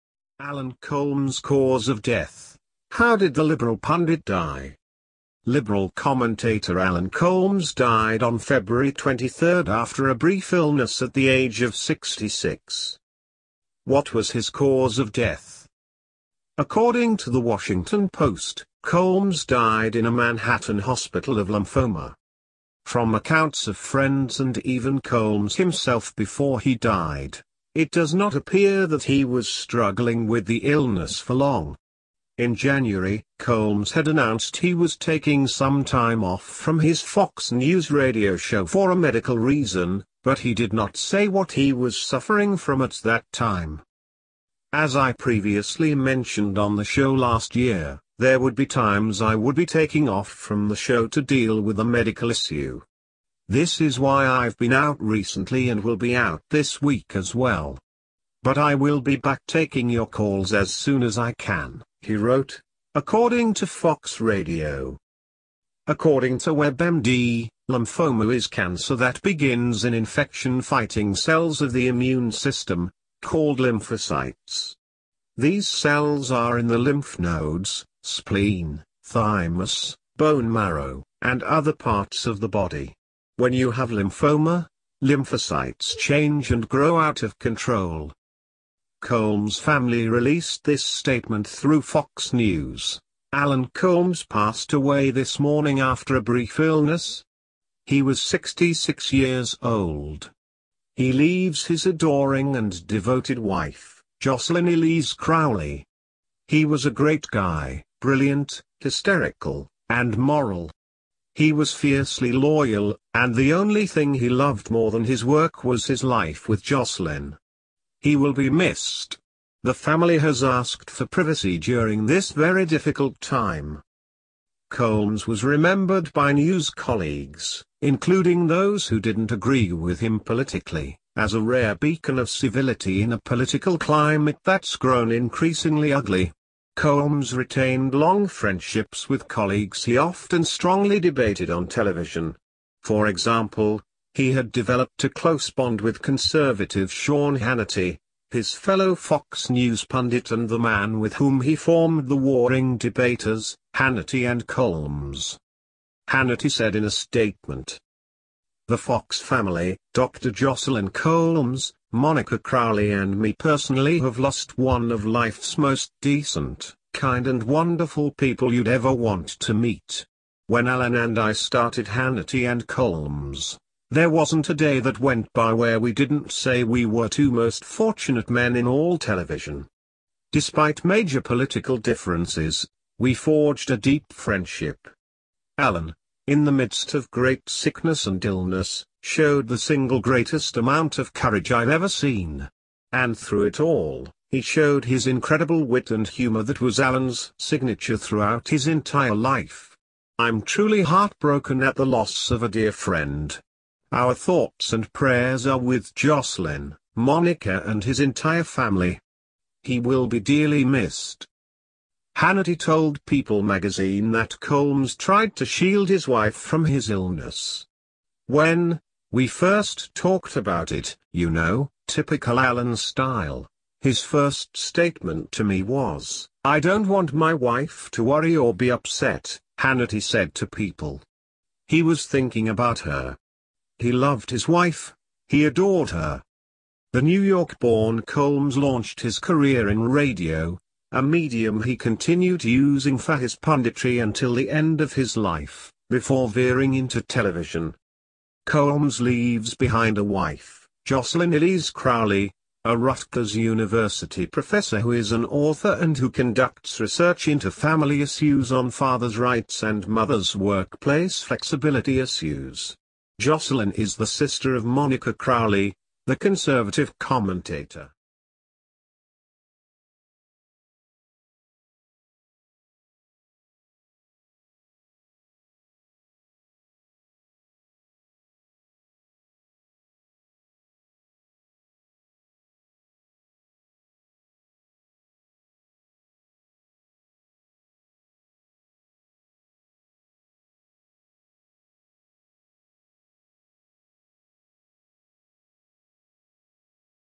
Alan Colmes Cause of Death: How Did the Liberal Pundit Die?
Liberal commentator Alan Colmes died on February 23 after a “brief illness” at the age of 66.

What was his cause of death?

According to The Washington Post, Colmes died in a Manhattan hospital of lymphoma.

From accounts of friends and even Colmes himself before he died, it does not appear that he was struggling with the illness for long. In January, Colmes had announced he was taking some time off from his Fox News radio show for a medical reason, but he did not say what he was suffering from at that time.

“As I previously mentioned on the show last year, there would be times I would be taking off from the show to deal with a medical issue. This is why I’ve been out recently and will be out this week as well. But I will be back taking your calls as soon as I can,” he wrote, according to Fox radio.

According to WebMD, “Lymphoma is cancer that begins in infection-fighting cells of the immune system, called lymphocytes. These cells are in the lymph nodes, spleen, thymus, bone marrow, and other parts of the body. When you have lymphoma, lymphocytes change and grow out of control.”

Colmes’ family released this statement through Fox News, “Alan Colmes passed away this morning after a brief illness. He was 66 years old. He leaves his adoring and devoted wife, Jocelyn Elise Crowley. He was a great guy, brilliant, hysterical, and moral. He was fiercely loyal, and the only thing he loved more than his work was his life with Jocelyn. He will be missed. The family has asked for privacy during this very difficult time.”